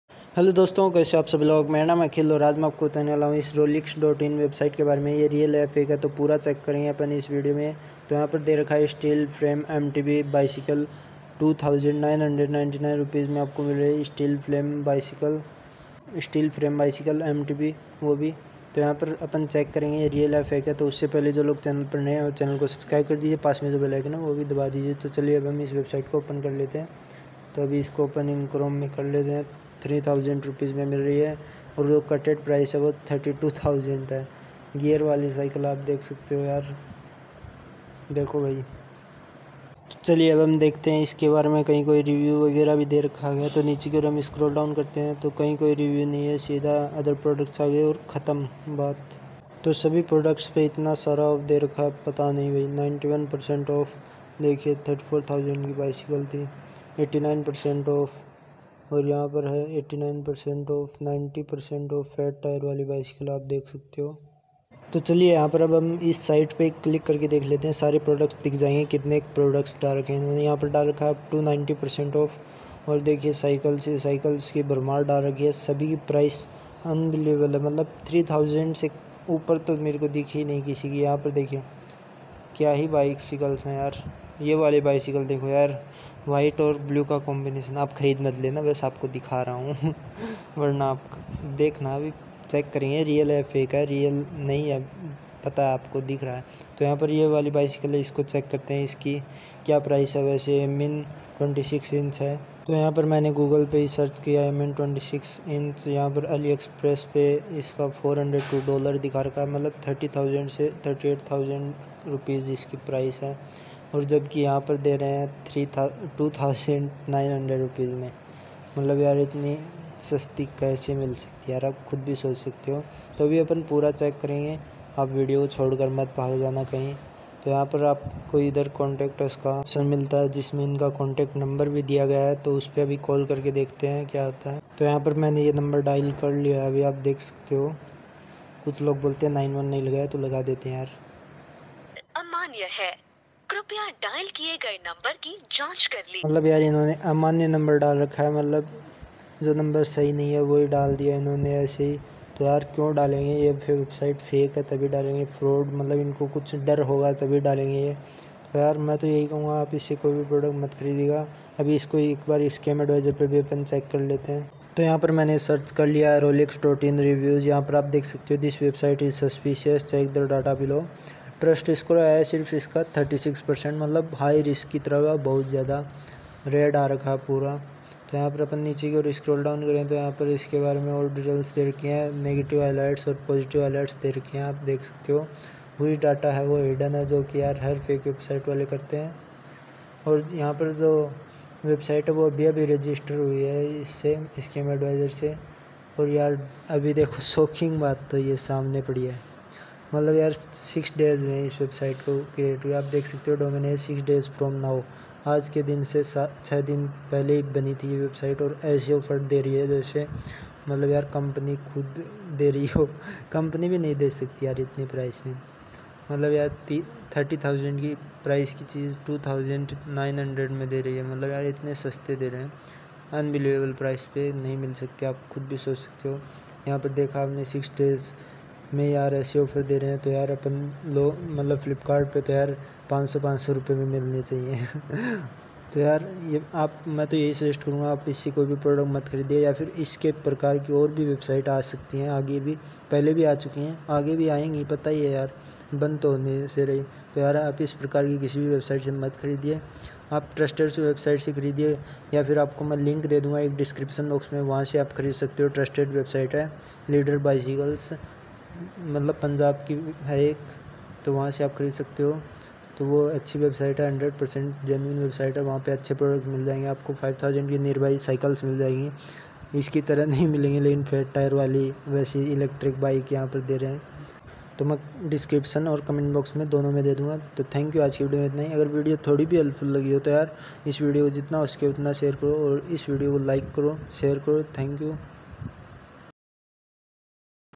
rollix in website review | rollix.in website real or fake | steel frame mtb for ₹2999😯
don't buy
this is fake website